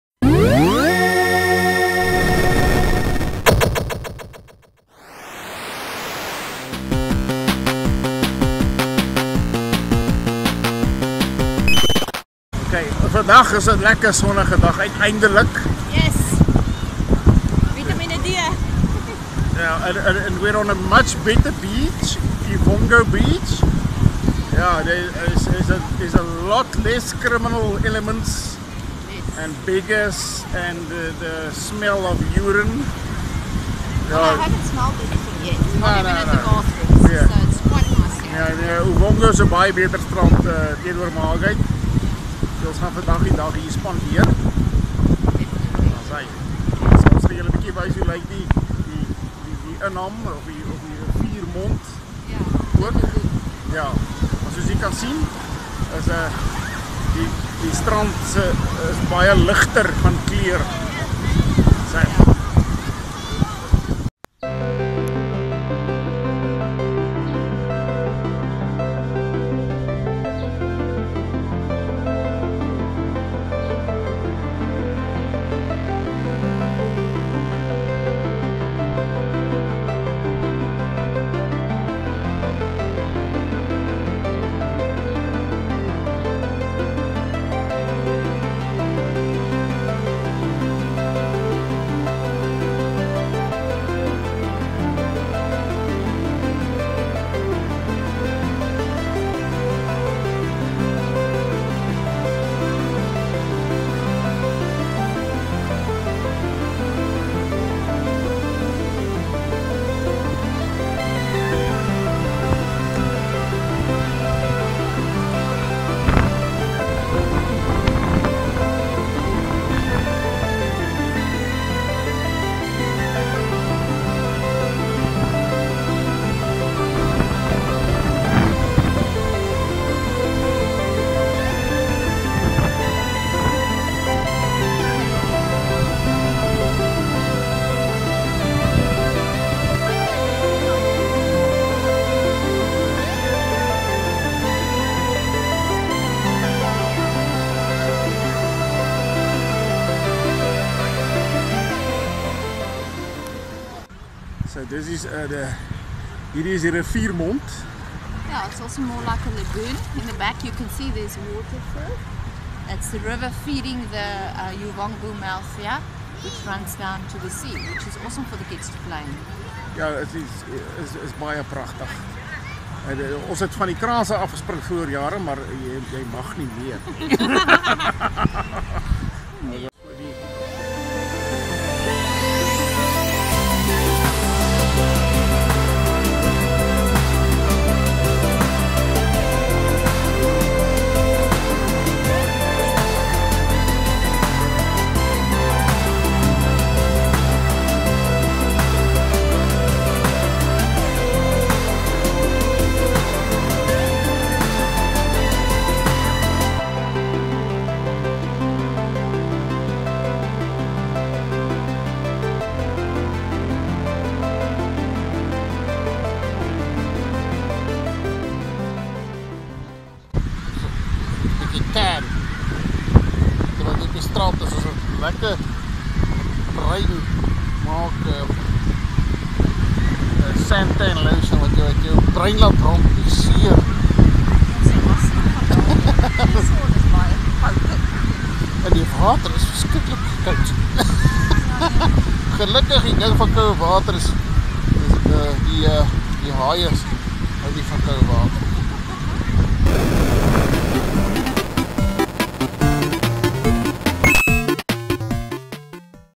South Coast Vacation Day 4/ Suid Kus Vakansie Dag 4
The Bat & The Cat is on Uvongo Beach!! Die Bat & Die Cat is op Uvongo Strand!!